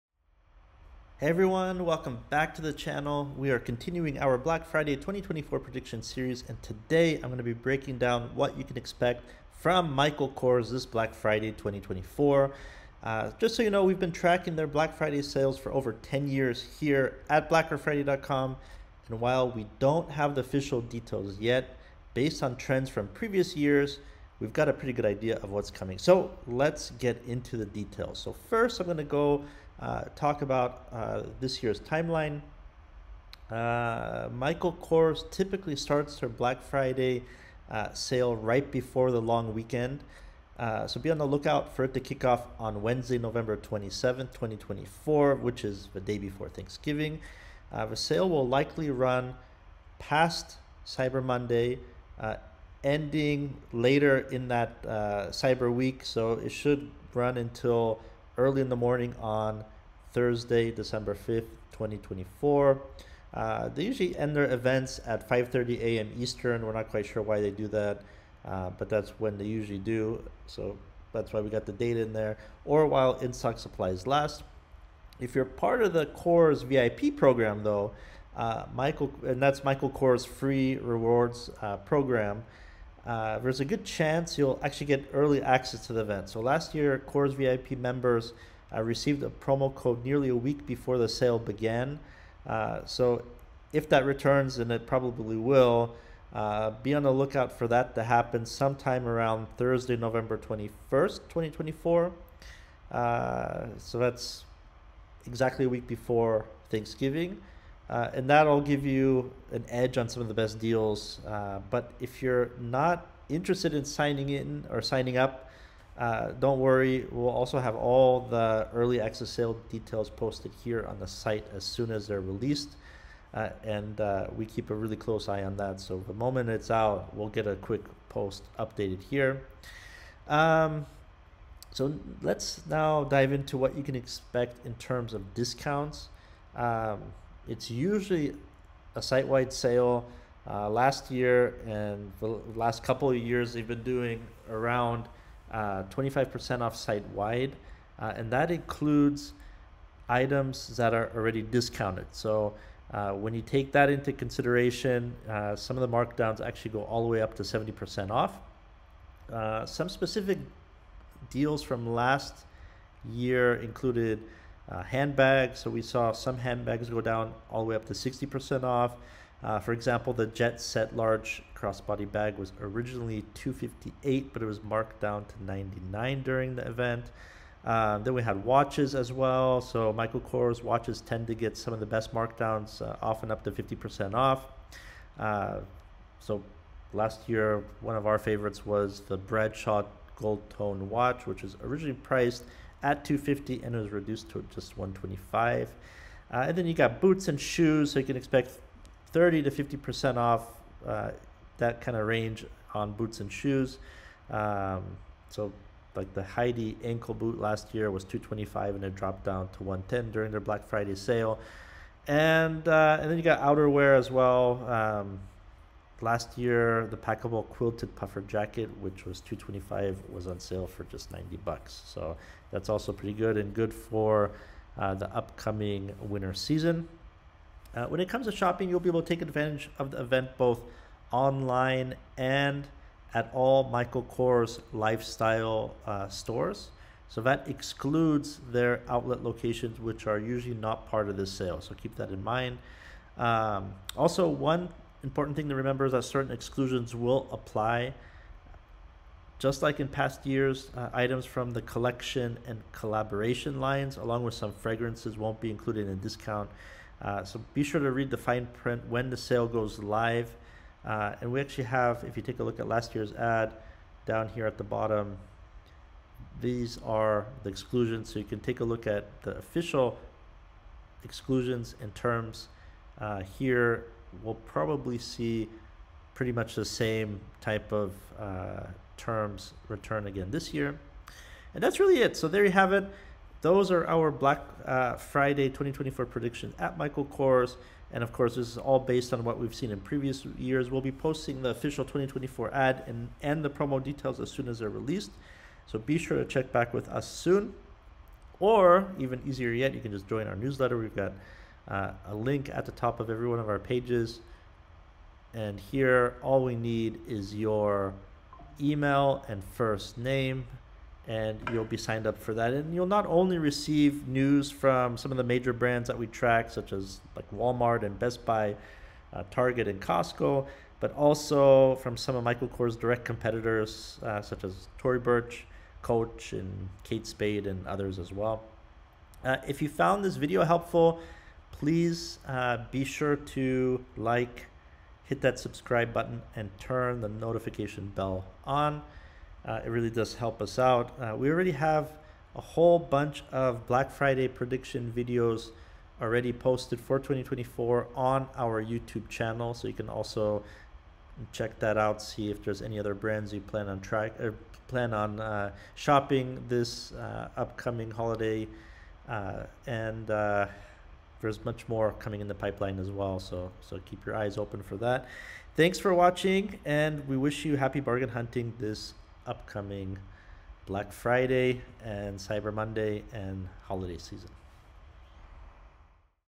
Michael Kors Black Friday 2024 Sale & Ad - What's Coming & When
In this video, I'll be discussing what you can expect from Michael Kors’ Black Friday 2024 sale this year.

In summary, based on trends from previous years, the sale will likely start on Wednesday, November 27, 2024, and run through Thursday, December 5, 2024, with early access for KORSVIP members potentially beginning on November 21st. As usual, exclusions will apply, with Collection and Collaboration lines likely not included, so check the fine print when the sale goes live.

for all the latest updates, including a copy of Michael Kors’ Black Friday 2024 ad, featured deals, sale dates and official terms. Happy bargain hunting!